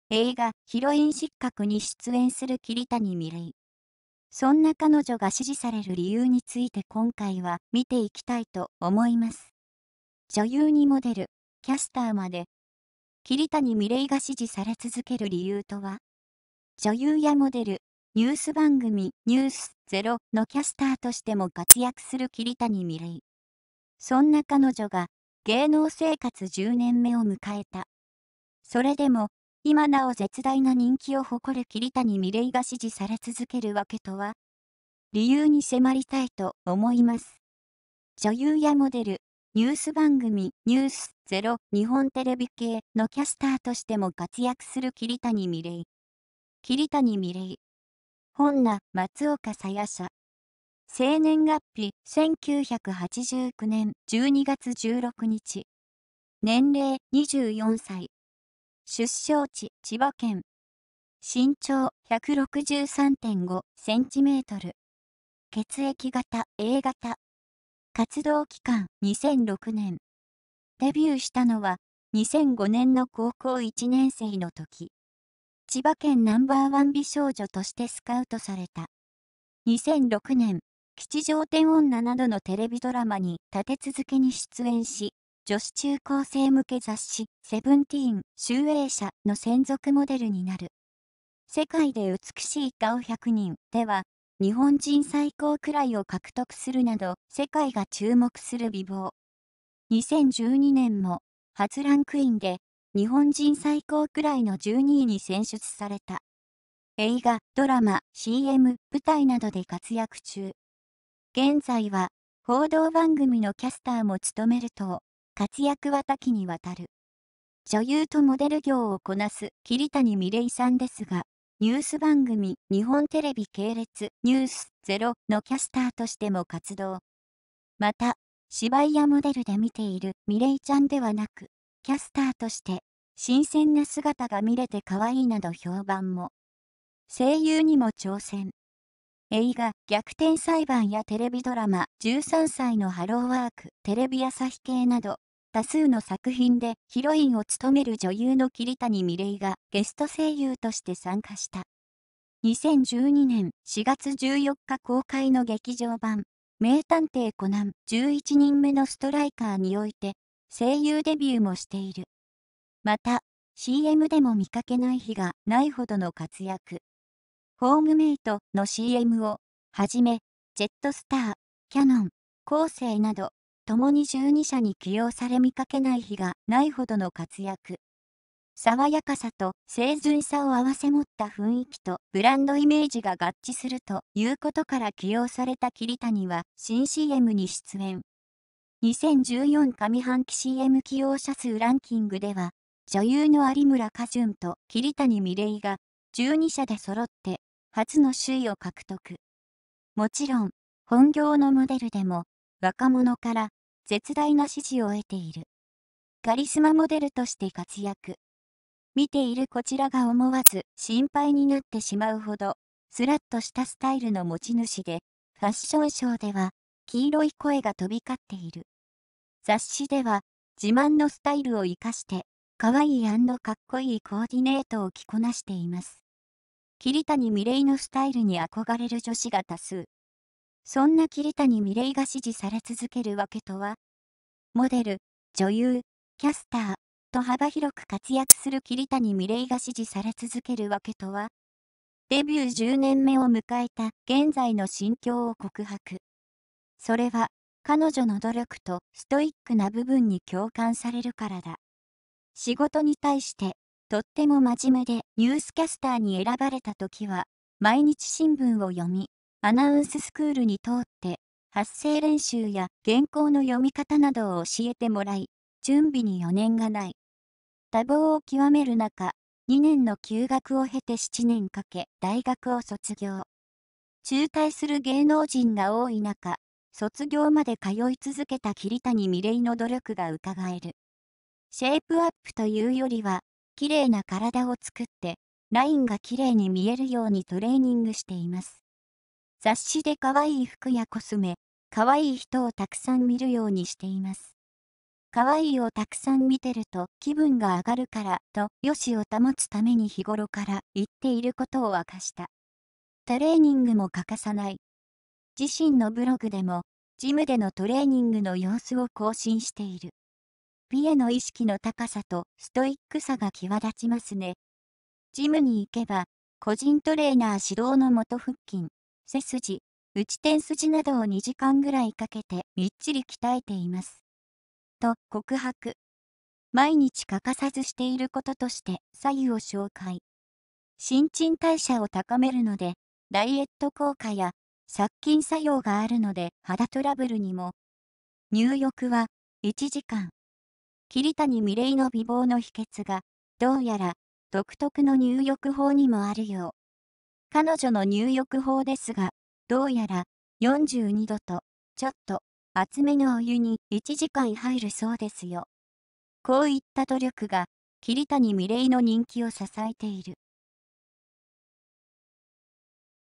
映画『ヒロイン失格』に出演する桐谷美玲が支持される理由とは!?
この動画では、映画『ヒロイン失格』に出演する桐谷美玲が支持される理由についてみていきます。